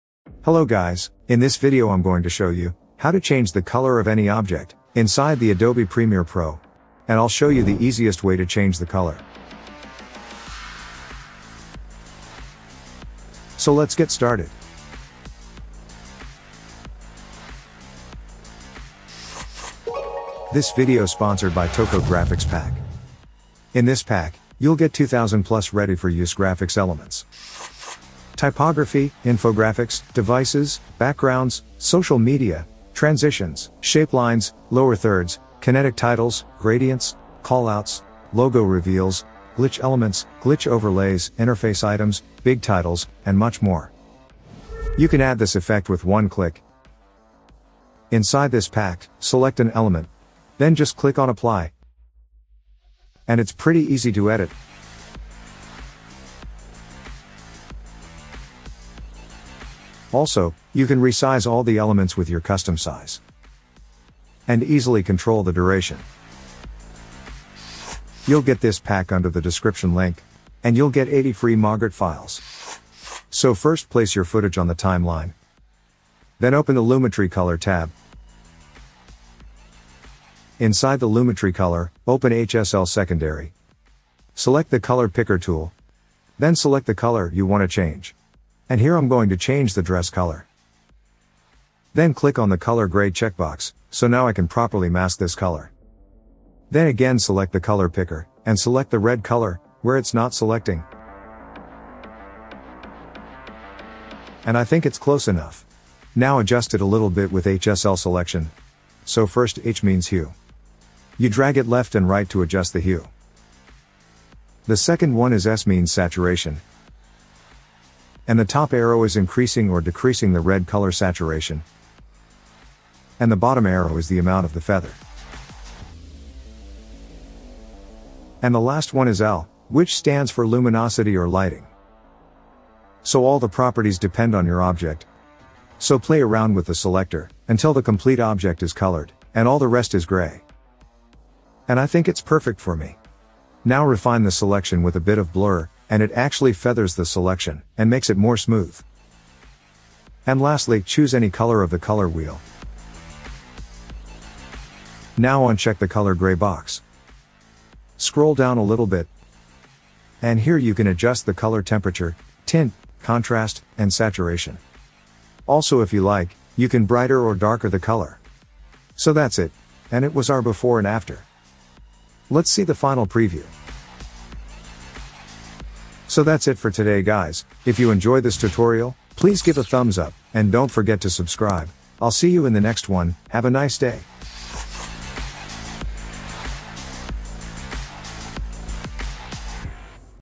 How to Change Color of an Object in Video | Premiere Pro CC Tutorial
➤In this video, I'll show you the best way to change the color of an object in your video, with the HSL Secondary tool inside the Lumetri color grading panel in Adobe Premiere Pro CC